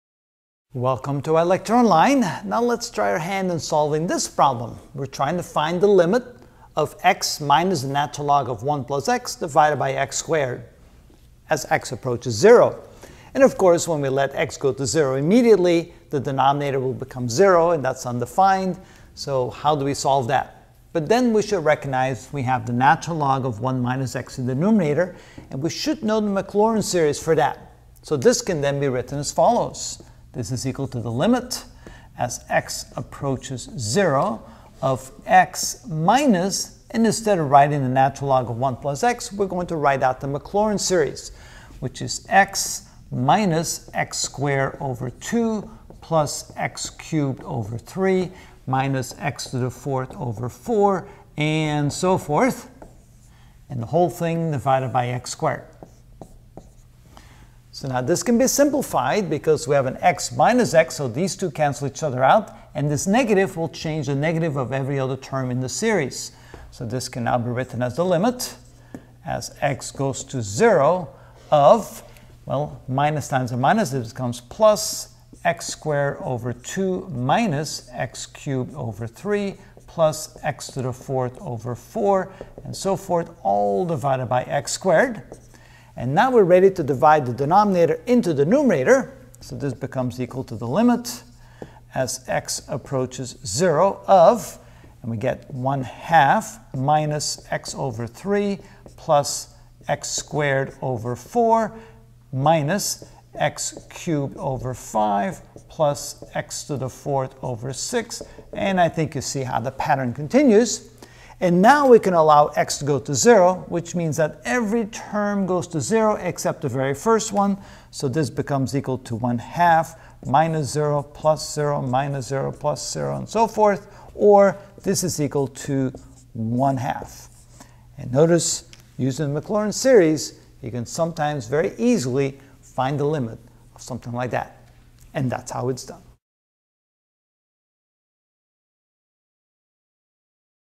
Calculus 2: Infinite Sequences and Series (77 of 86) Use the Maclaurin Series to Find Limit: 2
In this video I will use the Maclaurin series to find the limit of a function: example 2.